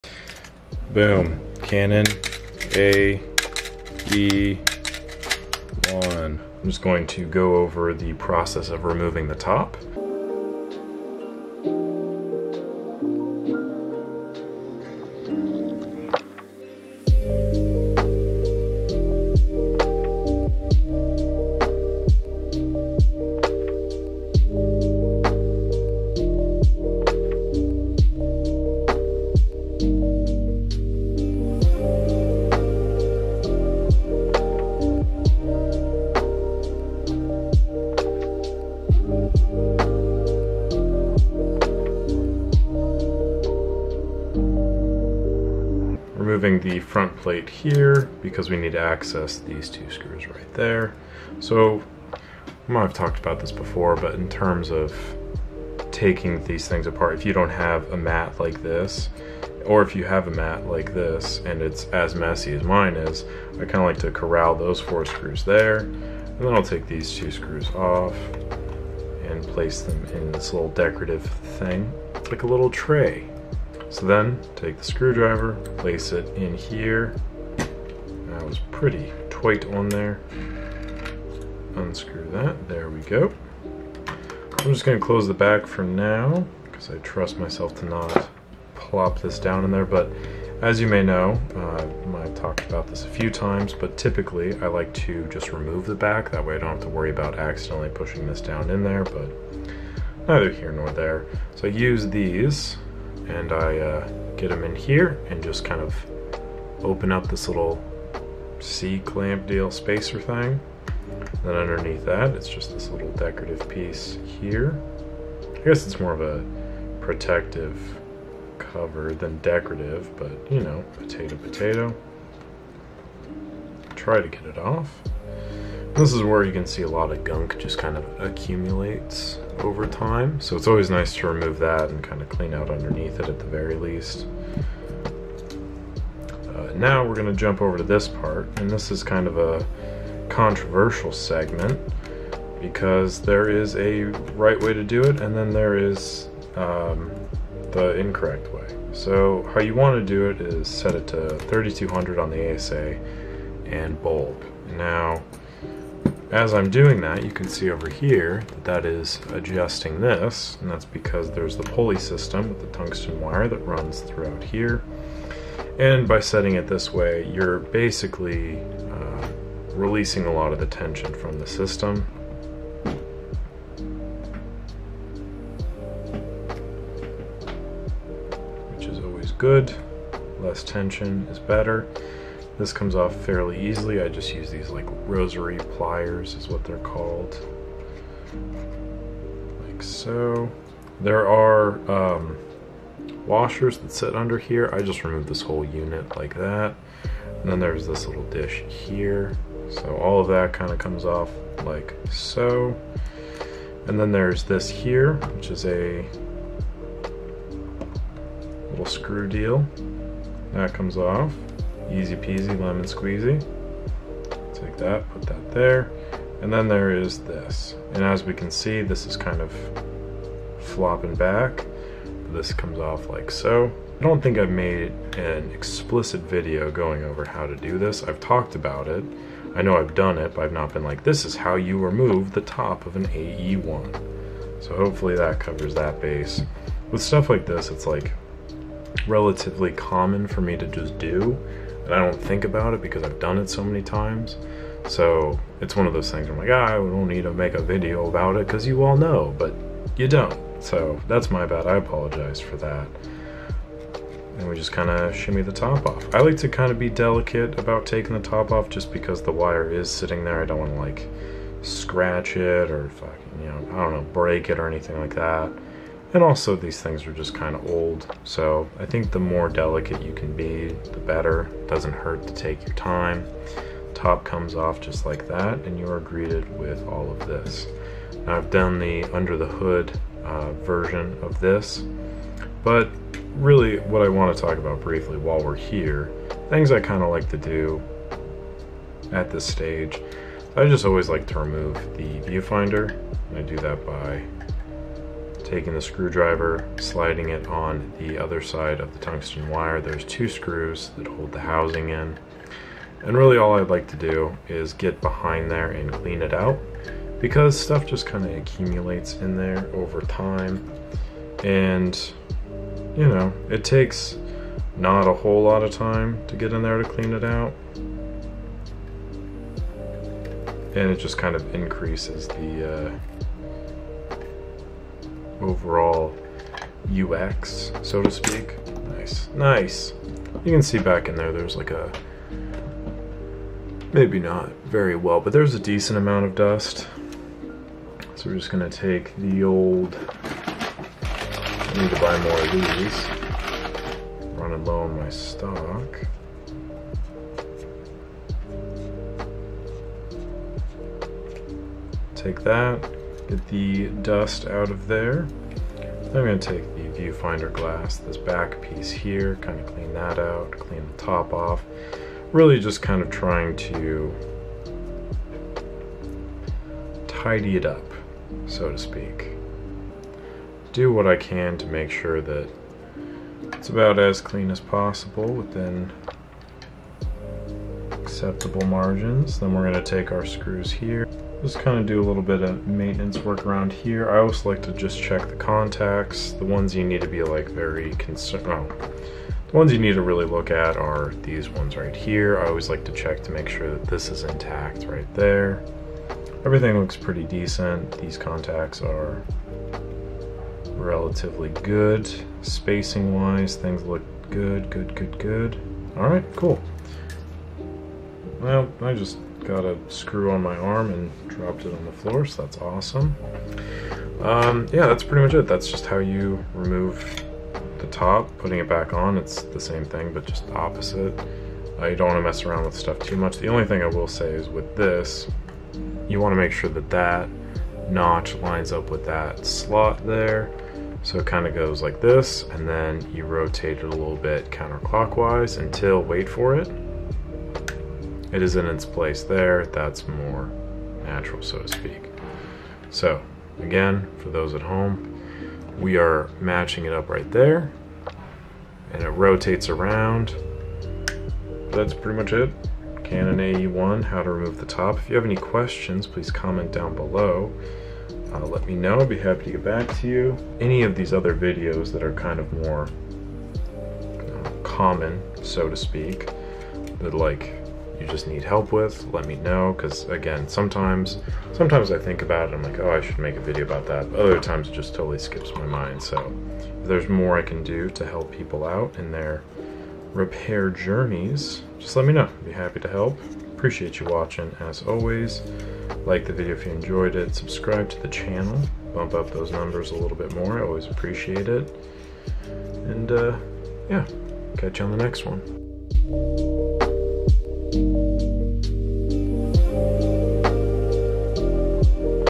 Removing the top of a Canon AE-1
How to take the top off a canon AE-1, relatively easy to do if you have the right tools and the needed space but I wouldn't recommend jumping into this unless you are fairly confident in your abilities.